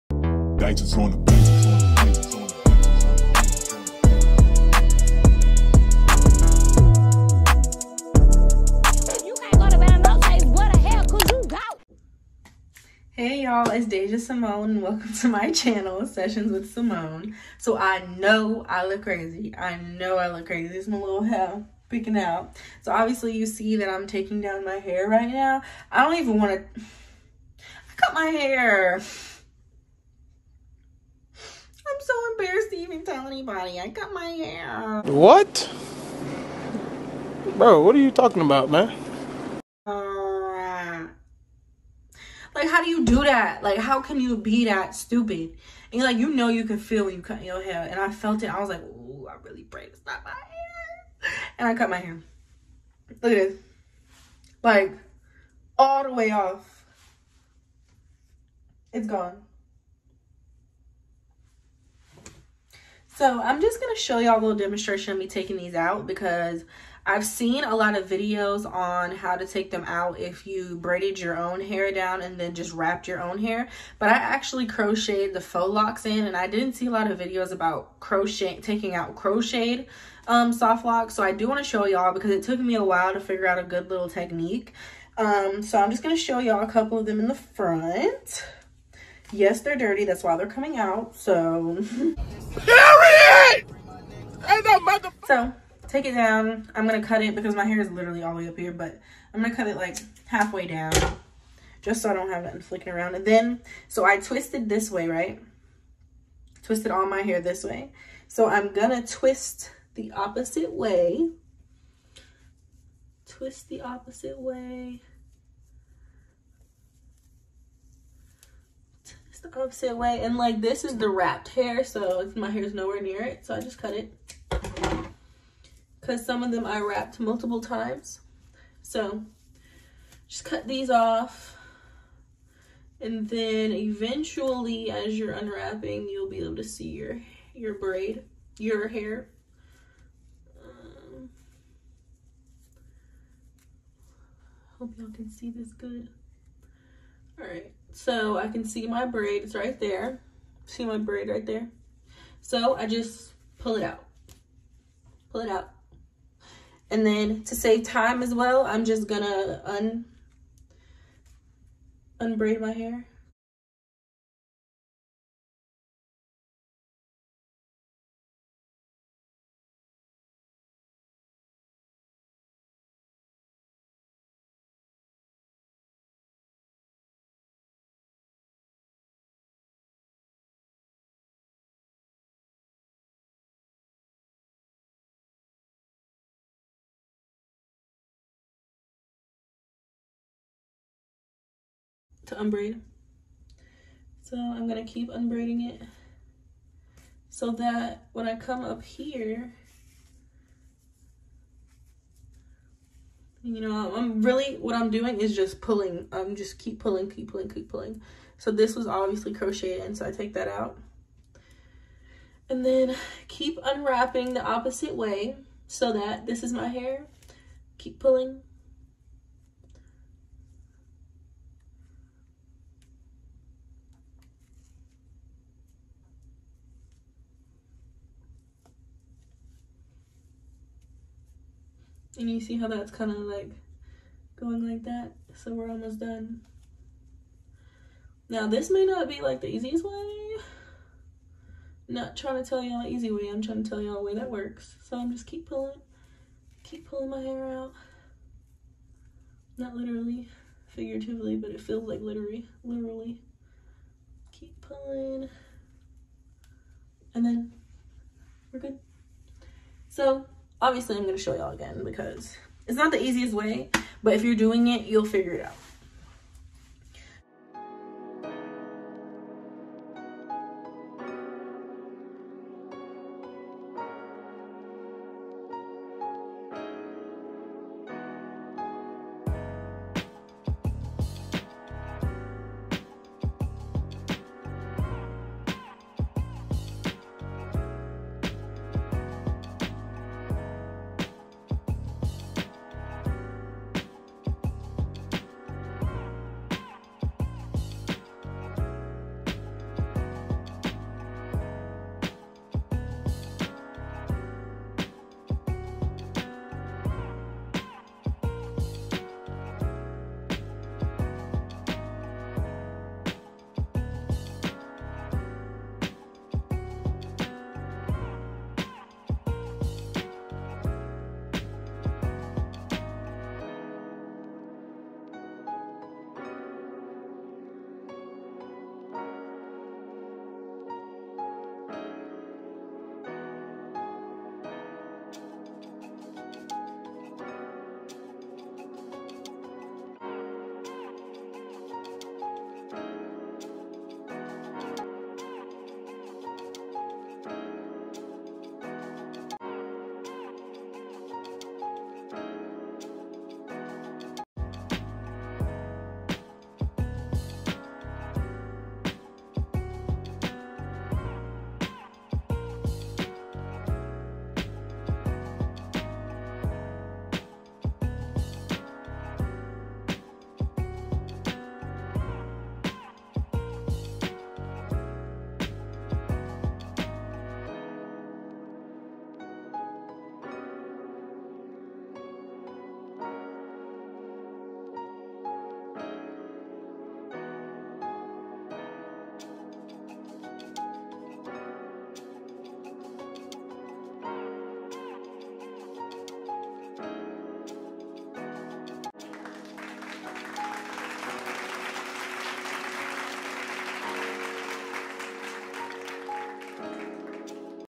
I CUT MY HAIR / How To Take Down Soft Locs
Hey yaaalllll!! I hope y'all enjoy this method of taking down soft locs. I tried to look up videos to help me take down soft locs quickly and I couldn't find the ones that were specifically crochet. So I had to make a video!! I hope y'all ENJOY xoxo

FAQ's:
Age: 20
Height: 5'6"
College: University of North Florida
Major: Multimedia Journalism
Minor: Political Science
Year: Junior

p.s. THIS VIDEO IS A PART OF MY 3 PART SERIES. I am doing a three part series on taking down soft locs, parting for a new set of soft locs, and installing soft locs. I hope y'all exciteddddd!!!